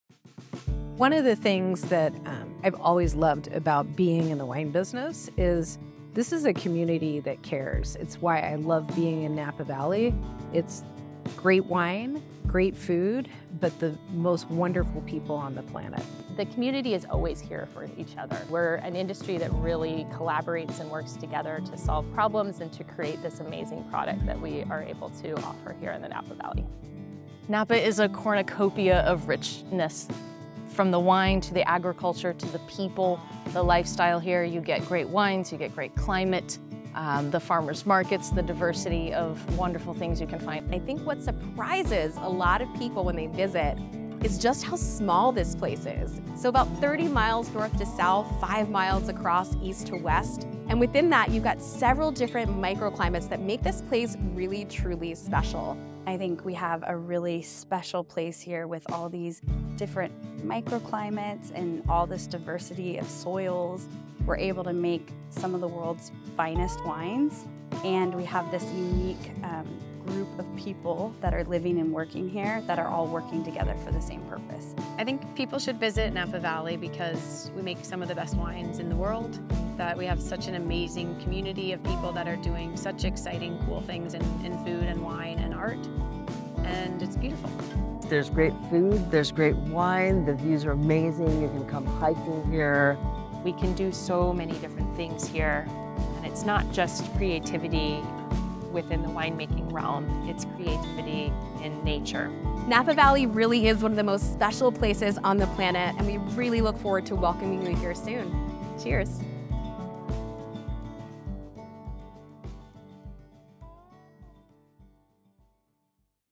Napa Valley: World Class Wines...and so much more!
People from all around the world flock to Napa to taste their world class wines but what exactly makes Napa Valley so special? Let's find out...